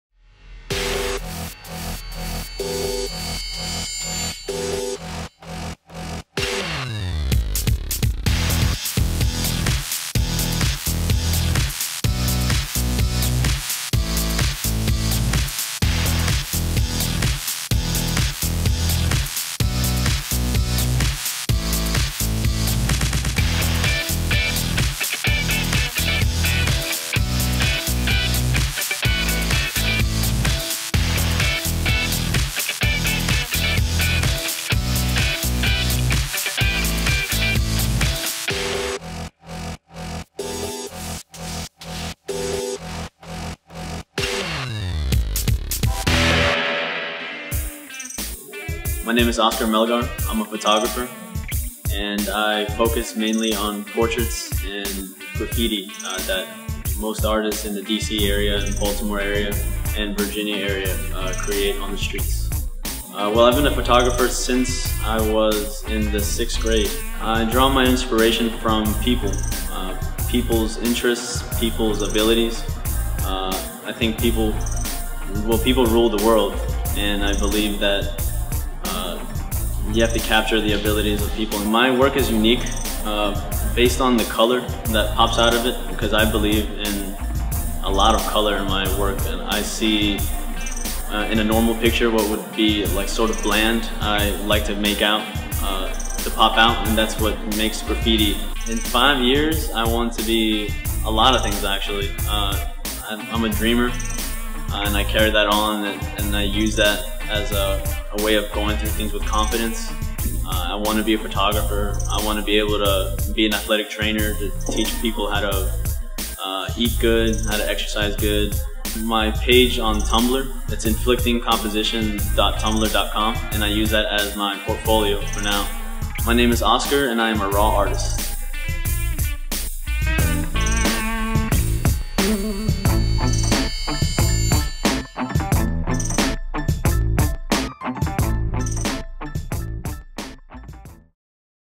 Oscar Melgar at Baltimore RAW: Provocations 10/4/12
Oscar Melgar showcases his work at the RAW Baltimore Provocations event.